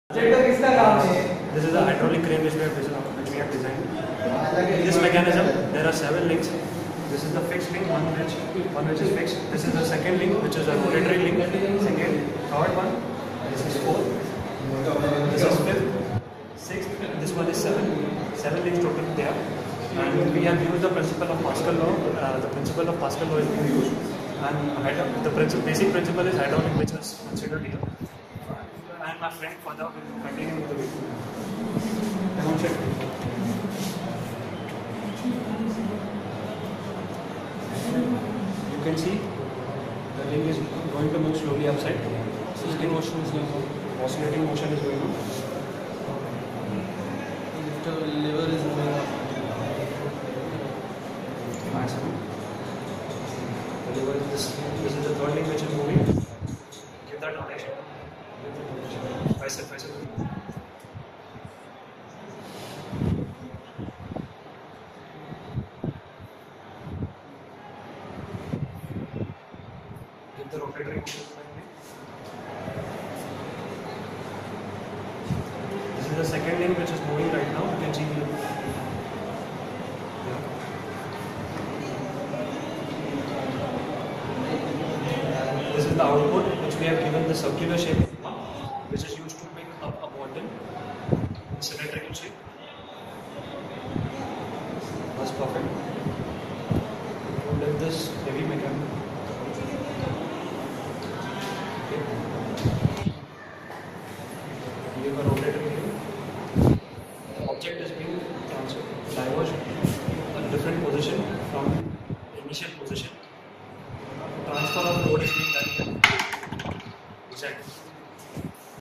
Hydraulic crane by MECHANICAL STUDENTS
Students made hydraulic crane using 7 link by using principle of pascal law